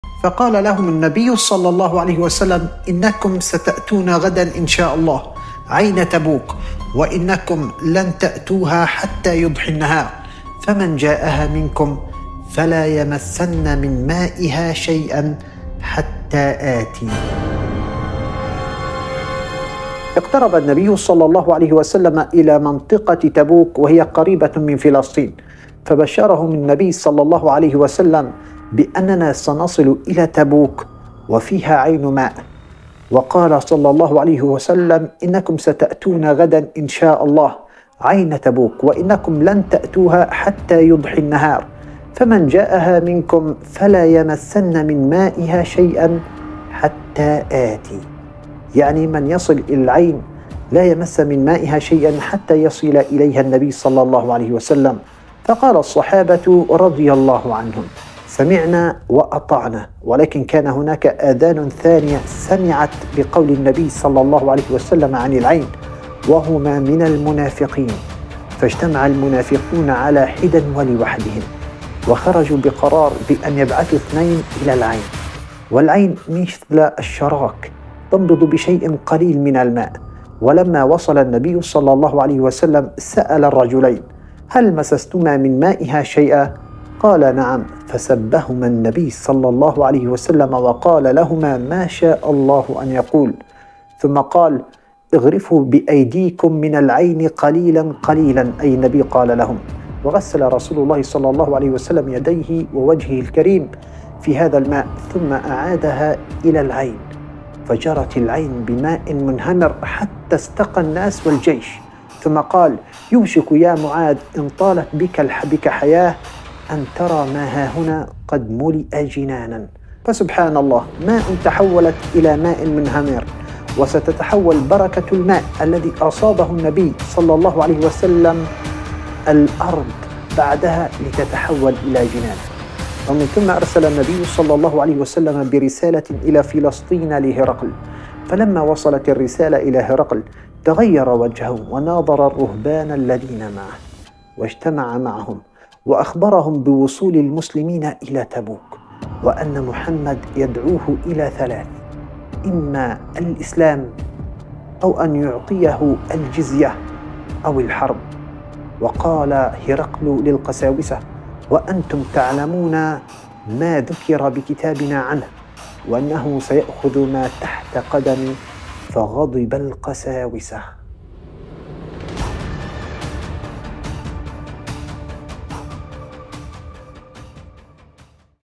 معجزه النبي في تبوك / غزوه تبوك ٤
غزوة تبوك هي واحدة من الغزوات التي شارك فيها النبي محمد صلى الله عليه وسلم خلال فترة الدعوة الإسلامية في القرن السابع الميلادي. وقعت غزوة تبوك في عام 630م، وهي واحدة من الغزوات الأخيرة التي شارك فيها النبي محمد صلى الله عليه وسلم.

تم التخطيط لغزوة تبوك بعد أن وردت معلومات استخباراتية تفيد بأن قوات الروم (البيزنطيين) يخططون لهجوم على المدينة المنورة. وبناءً على هذه المعلومات، قرر النبي محمد صلى الله عليه وسلم قيادة جيش المسلمين إلى تبوك لمواجهة هذا الاحتمال.

تشكلت قوة ضخمة من المسلمين للمشاركة في غزوة تبوك، وقد تطوع العديد من المسلمين للانضمام إلى الجيش حتى وإن كان ذلك يعني ترك أعمالهم وأسرهم. ومع ذلك، فإن القوات الرومية لم تتقدم لمواجهة الجيش المسلم، وبالتالي لم يحدث صراع عسكري فعلي.

وعندما وصل جيش المسلمين إلى تبوك، تم توقيع معاهدة سلام مع القبائل المحلية وعرض الإسلام عليها. وبعد إقامة صلاة الجمعة في تبوك، عاد الجيش المسلم إلى المدينة المنورة.

تعد غزوة تبوك من أبرز الغزوات التي تعلم منها المسلمون العديد من الدروس والعبر، وتعد مثالًا قويًا على الوحدة والتضحية من أجل دفاع الدين والمجتمع.
من معجزات النبي | غزوه تبوك ٣